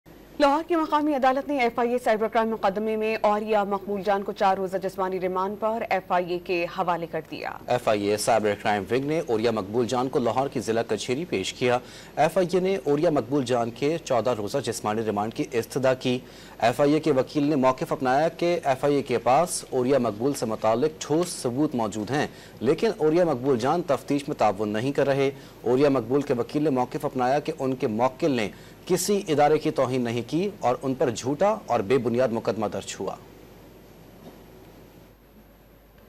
Orya Maqbool Jaan handed over to FI on 4-day physical remand| Dawn News
اوریا مقبول جان 4 روزہ جسمانی ریمانڈ پر ایف آئی کے حوالے